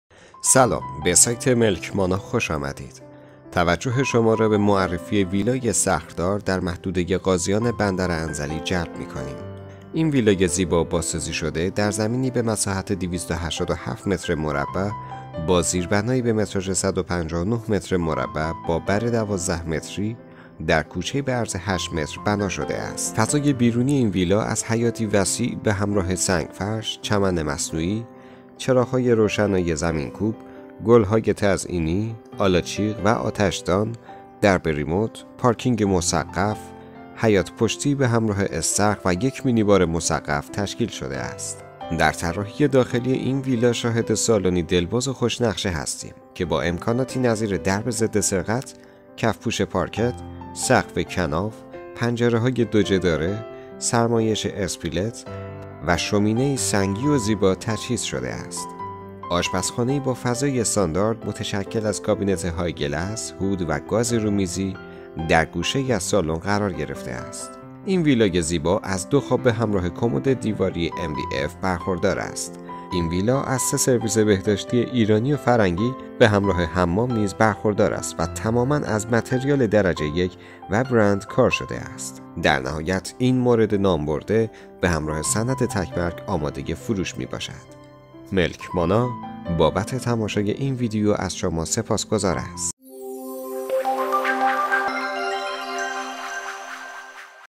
ویلا مدرن 287 متری در معلم غازیان بندرانزلی
برای اطلاع از قیمت واقعی ویلا و آپارتمان در خیابان معلم بندرانزلی روی لینک زیر کلیک کنید!

شناسه ملک: A4697
مساحت ملک: 159 متر
مساحت زمین: 287 متر

برای مشاهده ملک های بیشتر و دیدن تور مجازی ملک ها میتوانید به سایت اصلی ملک مانا مراجعه کنید.
شماره تماس ویژه 4 رقمی ملک مانا، سامانه رسمی املاک شمال کشور!